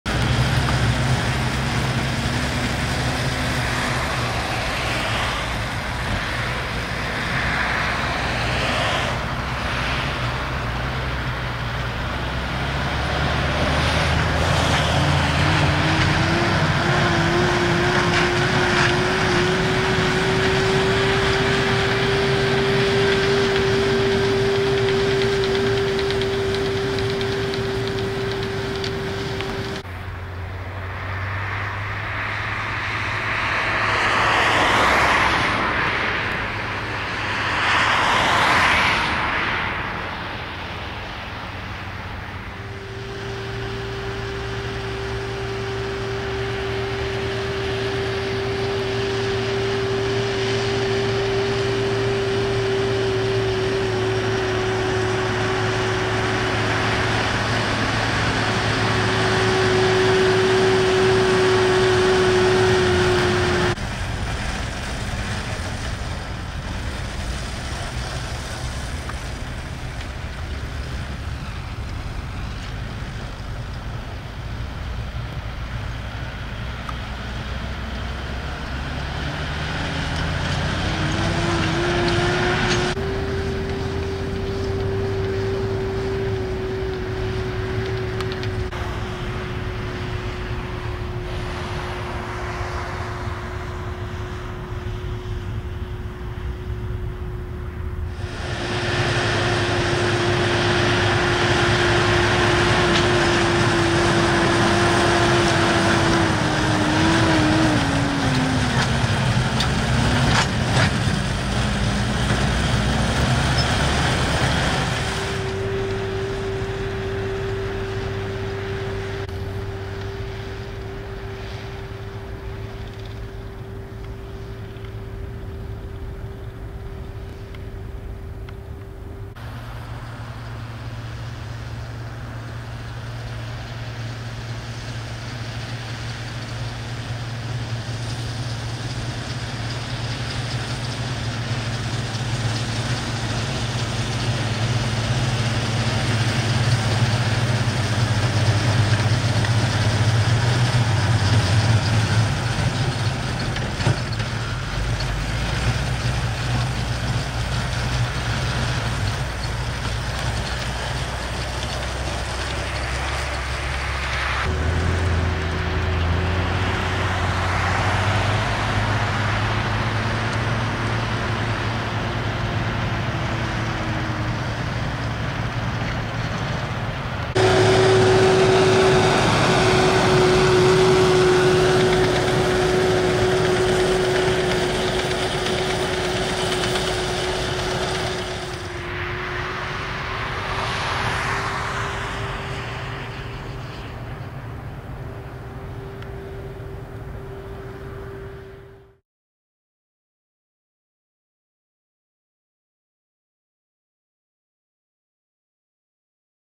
NEW HOLLAND TM AND T6000 SERIES TRACTORS AND SPREADERS
FROM THE EUROPEAN ARCHIVES: Sascha Jussen's footage of contractors working with New Holland TM and T6000 Series tractors and Bergmann spreaders